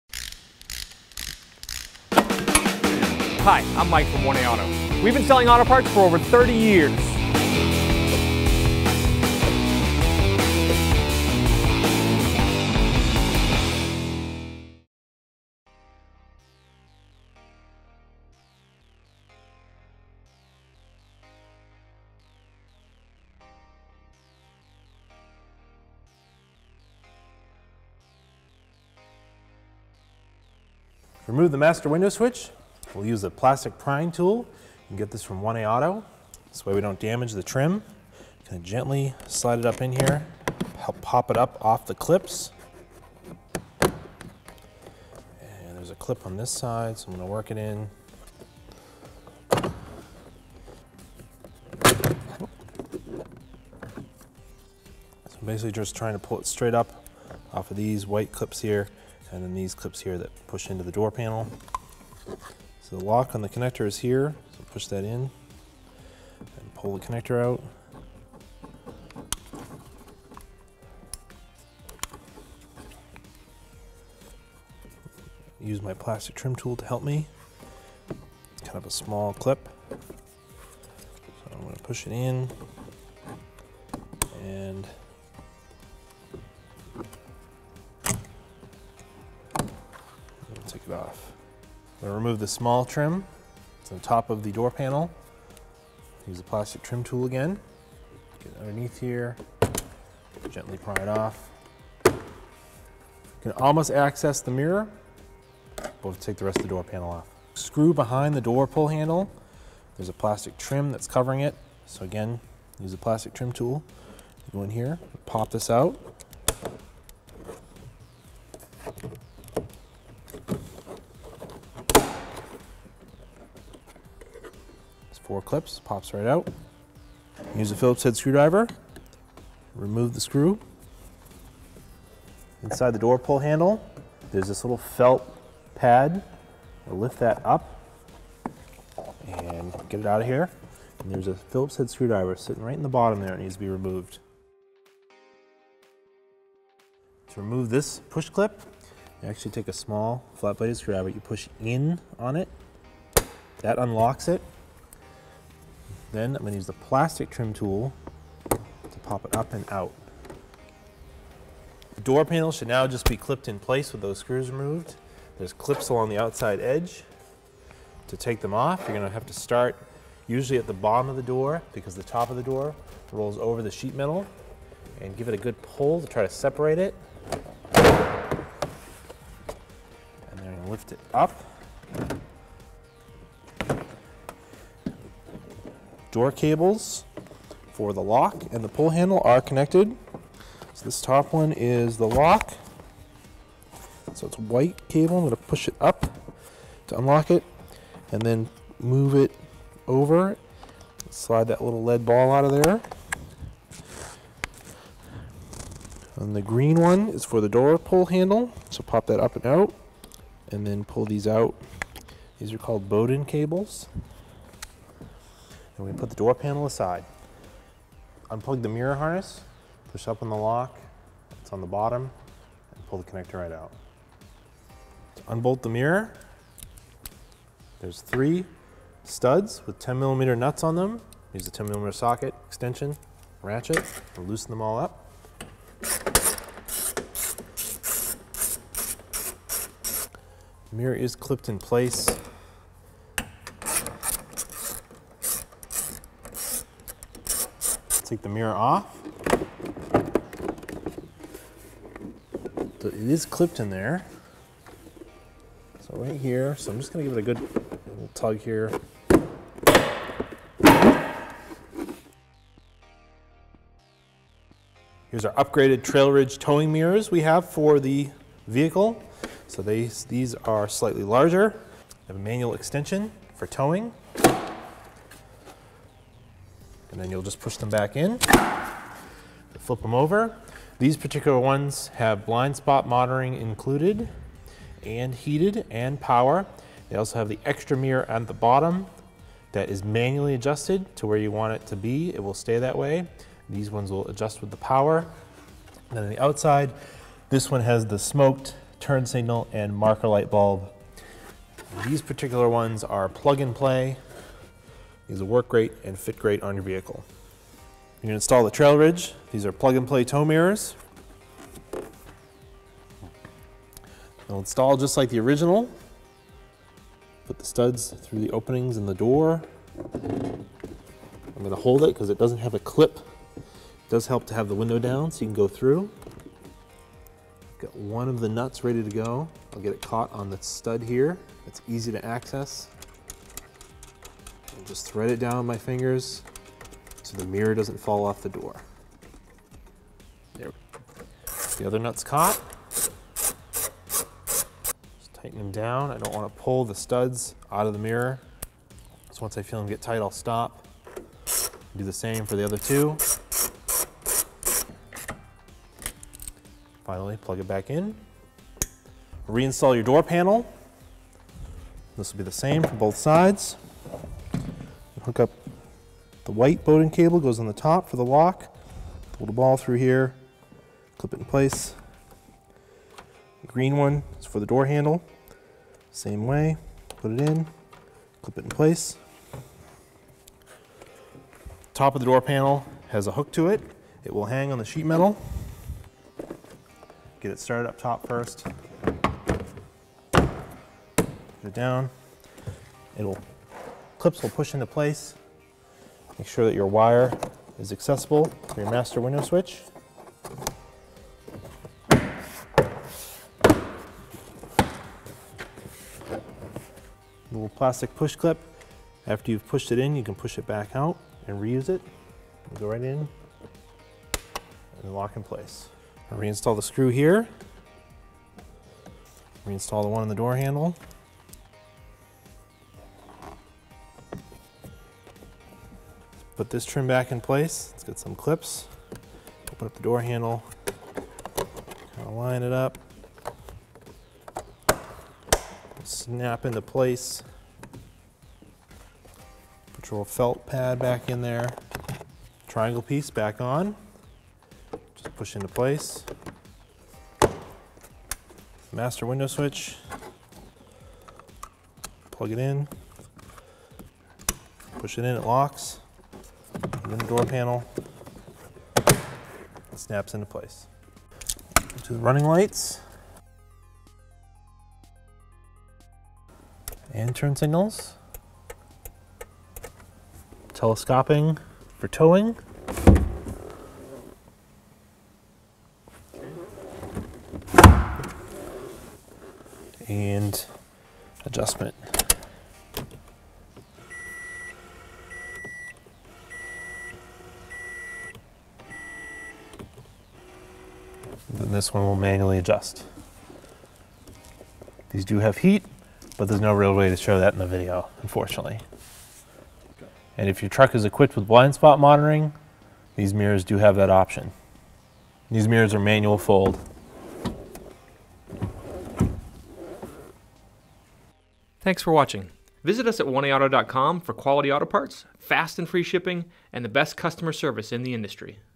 How to Install Trail Ridge Plug & Play Tow Mirrors 14-16 Toyota Tundra
1A Auto shows you how to install Trail Ridge plug and play upgrade towing mirrors. This video is applicable to the 14, 15, 16 Toyota Tundra.